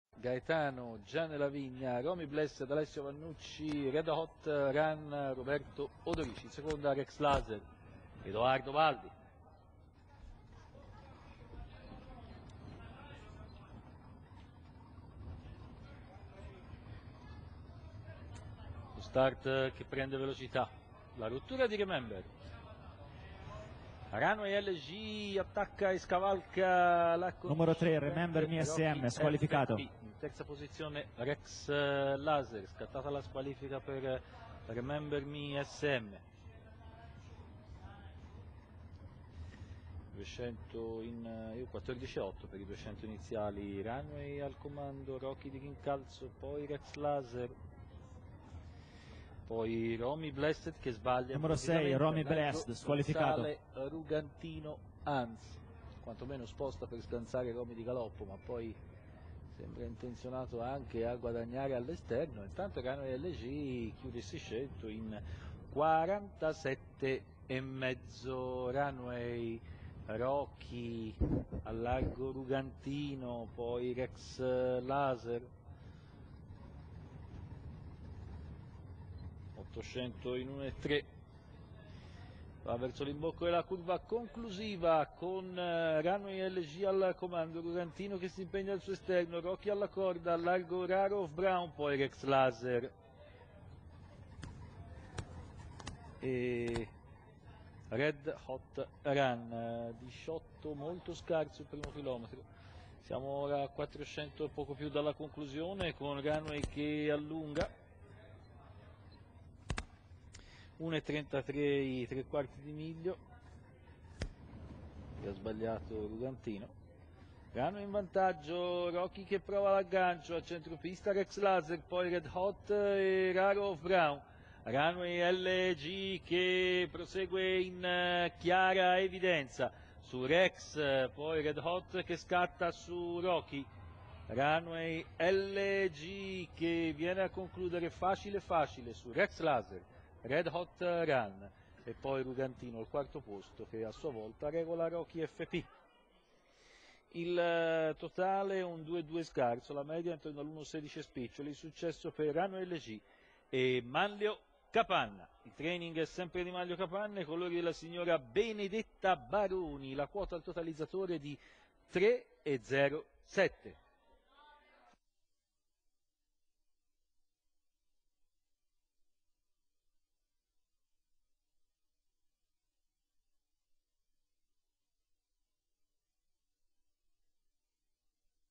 23/09/2013, Follonica , Premio Suvereto
1. RUNAWAY LG (CAPANNA M.), 2. REX LASER (BALDI EDOARDO), 3. RED HOT RUN (ODORICI ROBERTO), 4. RUGANTINO ANS (CICOGNANI GLAUCO), 5. ROCKY FP (DI NICOLA M.), 6. RARO OF BROWN (GIANNELAVIGNA G.), 99. REMEMBER ME SM (BELLEI E.), 99. ROMY BLESSED (VANNUCCI ALESSIO)